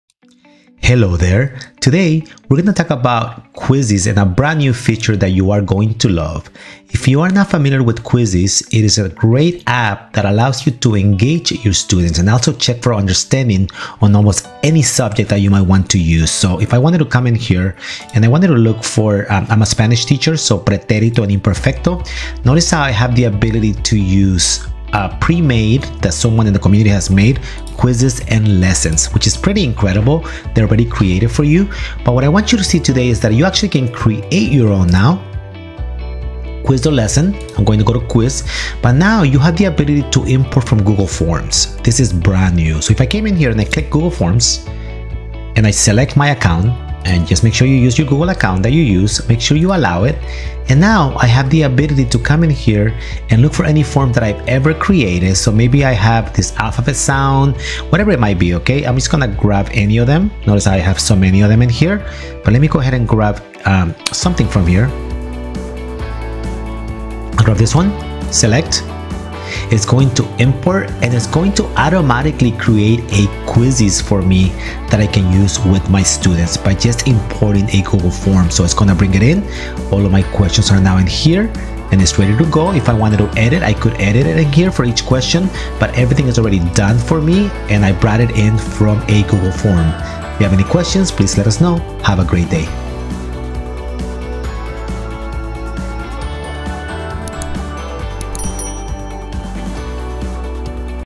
Import Google Forms to Create Quizizz
Import Google Form questions inot Quizizz to create your games. If you are using Forms as formative, you can now gamify those activities.